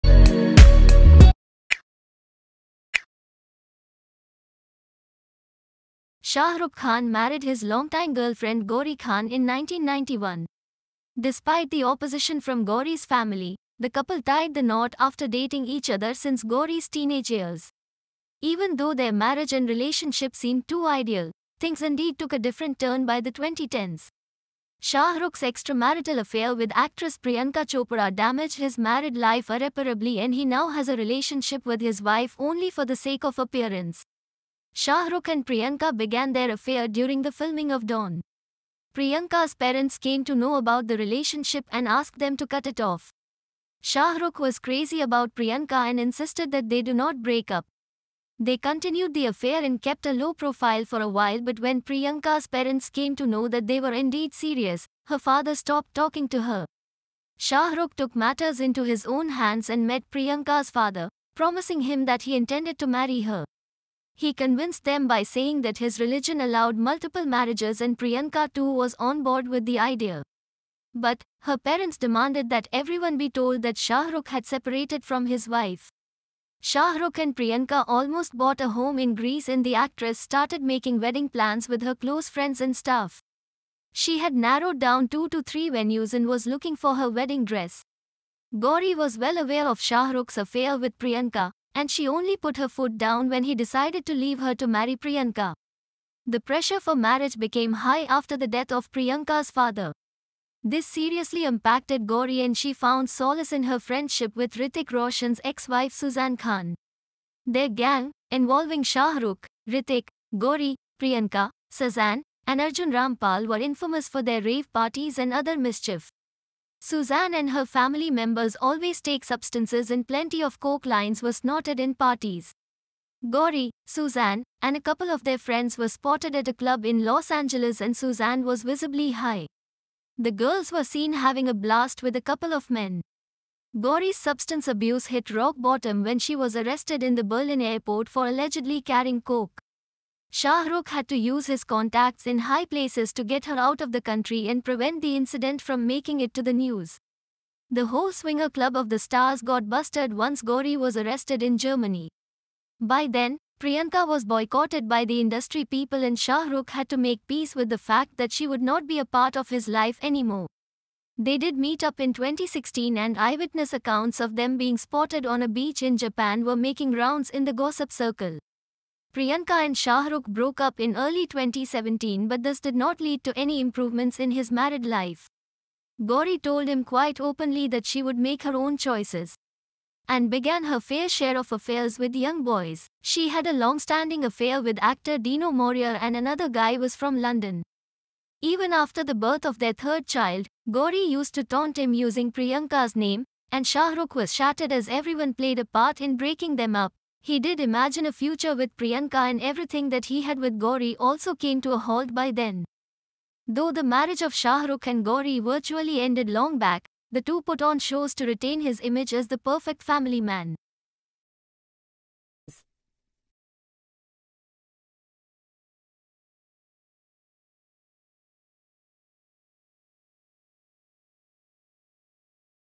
Shah Rukh Khan's Relationship SECRETS with Gauri Khan & Priyanka Chopra - 5 Facts
5 Facts About Shah Rukh Khan's Marriage & Link with Priyanka Chopra

The video presents some lesser-known facts about Shah Rukh Khan and his wife Gauri Khan’s marriage and his affair with Priyanka Chopra.